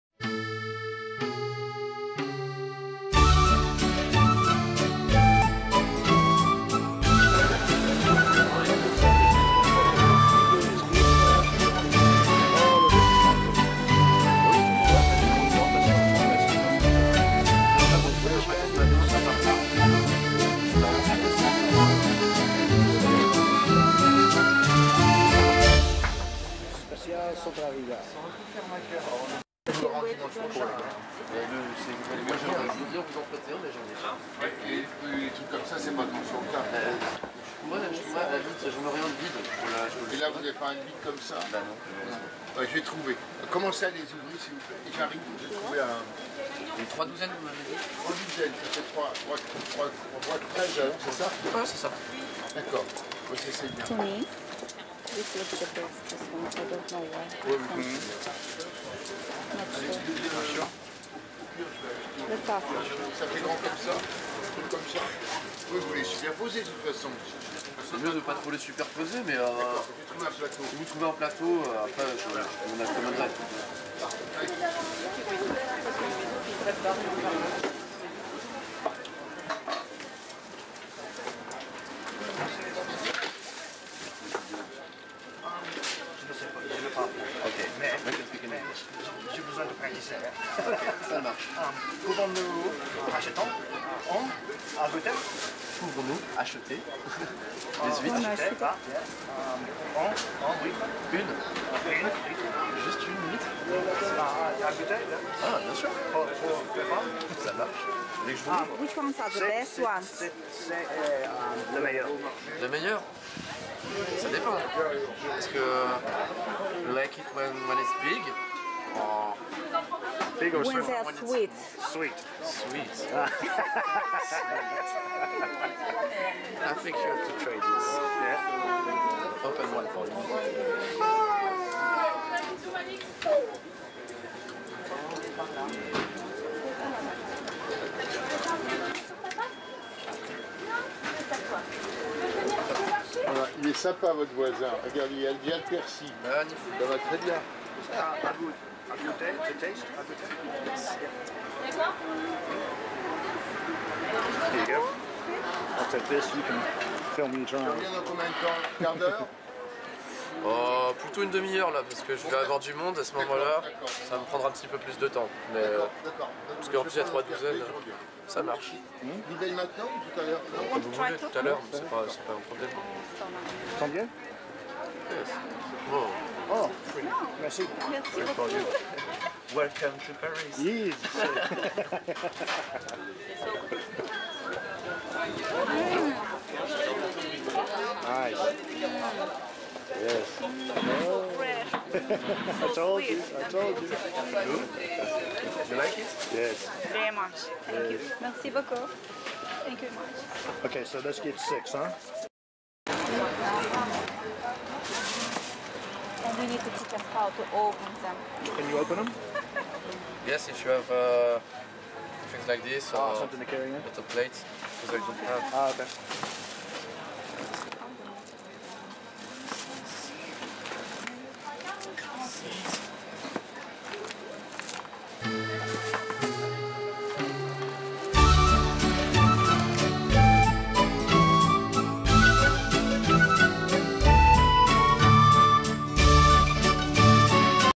Vincennes, Paris Food Markets, Tasting and Buying Fresh French Oysters (Les Huitres)
Vincennes, Paris Food Markets, Tasting and Buying Fresh Oysters (Les Huitres).  The oyster guy is helping me with my pronunciation of my limited French vocabulary and helps us choose the "sweetest" oysters available...all oysters brought in fresh on the day!